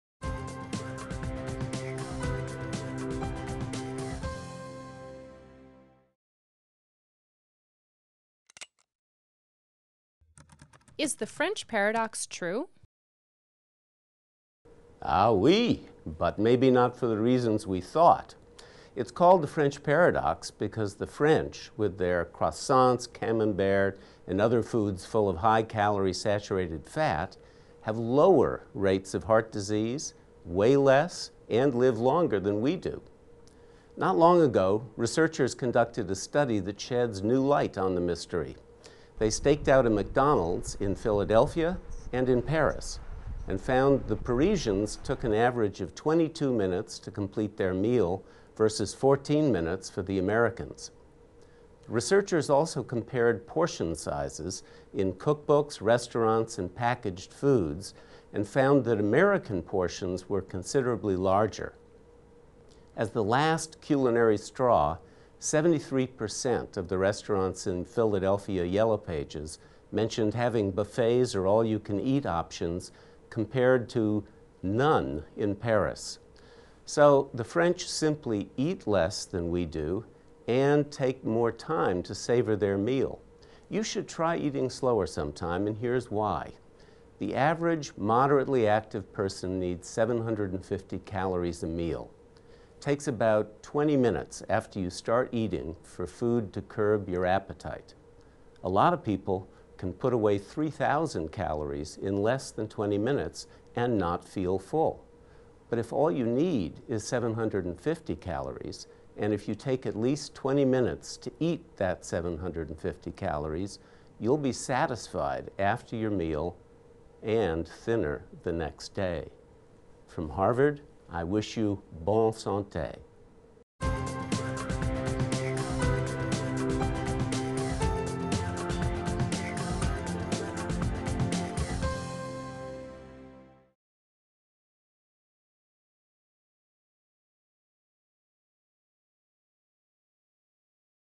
French Diet Paradox Solved
Harvard Medical School video explains why the French diet is healthier